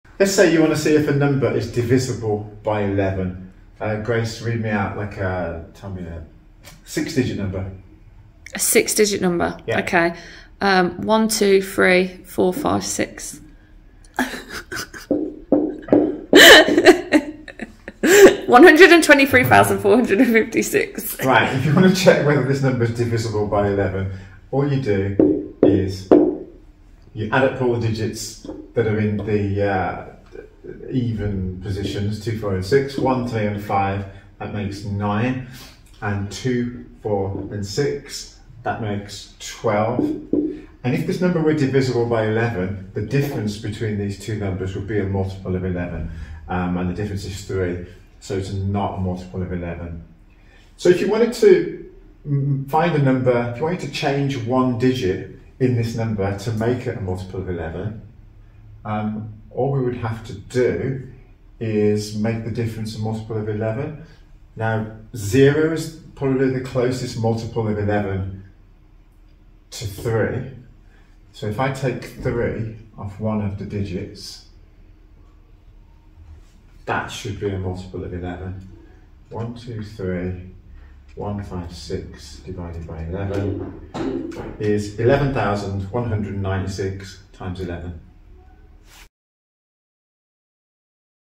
Is this a multiple of 11?
A quick way to tell if a number is a multiple of 11 or not.